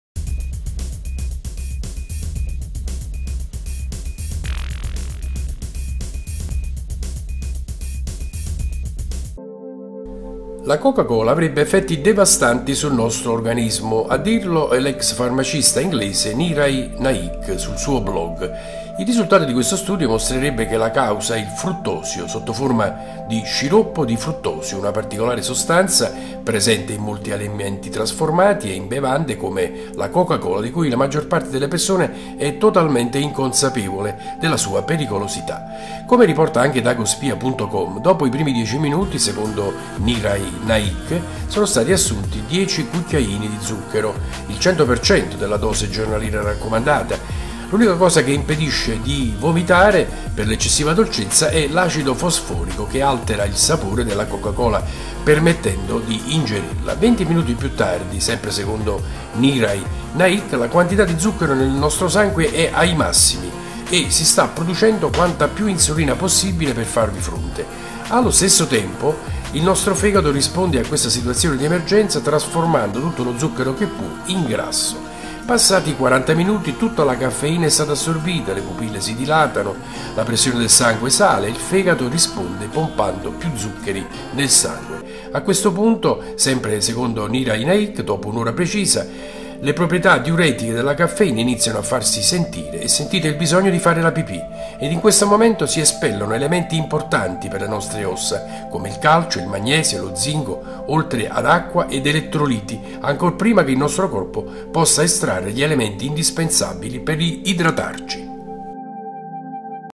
Quotidiano Medicina News 31/7/15. Coca Cola, i suoi effetti devastanti secondo farmacista inglese
Coca Cola, its devastating effects after 60 minutes, according to a pharmacist English.
La Coca-Cola avrebbe effetti devastanti sul nostro organismo. A dirlo è l’ex farmacista inglese Niraj Naik sul suo blog. Il risultato di questo studio mostrerebbe che la causa è “il fruttosio sotto forma di sciroppo di fruttosio, una particolare sostanza presente in molti alimenti trasformati e bevande” come la Coca-Cola della cui pericolosità “la maggior parte delle persone è totalmente inconsapevole. Come riporta anche Dagospia.com, dopo i primi 10 minuti “sono stati assunti 10 cucchiaini di zucchero (il 100% della dose giornaliera raccomandata).  L’unica cosa che ci impedisce di vomitare per l’eccessiva dolcezza è l’acido fosforico che altera il sapore della Coca-Cola, permettendoci di ingerirla”. Venti minuti più tardi “la quantità di zucchero nel nostro sangue è ai massimi e stiamo producendo quanta più insulina possibile per farvi fronte. Allo stesso tempo il nostro fegato risponde a questa situazione di emergenza trasformando tutto lo zucchero che può in grasso”. Passati quaranta minuti “tutta la caffeina è stata assorbita, le pupille si dilatano, la pressione del sangue sale e il fegato risponde pompando più zucchero nel sangue”. A questo punto dopo un’ora precisa ecco, secondo Naik, che “le proprietà diuretiche della caffeina iniziano a farsi sentire e sentiamo il bisogno di fare la pipì. In questo momento espelleremo elementi importanti per le nostre ossa come calcio, magnesio e zinco, oltre a acqua ed elettroliti ancor prima che il nostro corpo potesse estrarre gli elementi indispensabili per idratarci”

(liberamente tratto da Il Fatto Quotidiano)